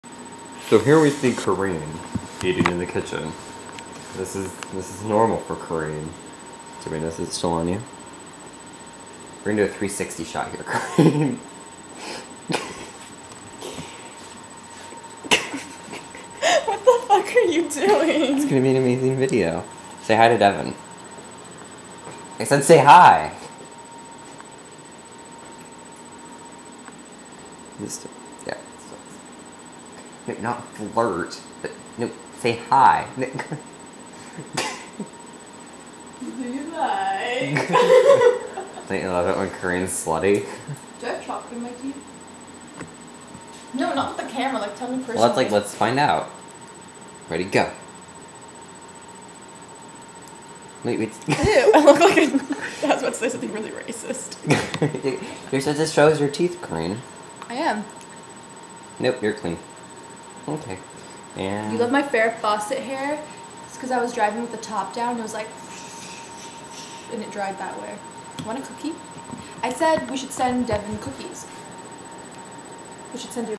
Cookie Baking With Kareen
Kareen, Me, and a boring Sunday afternoon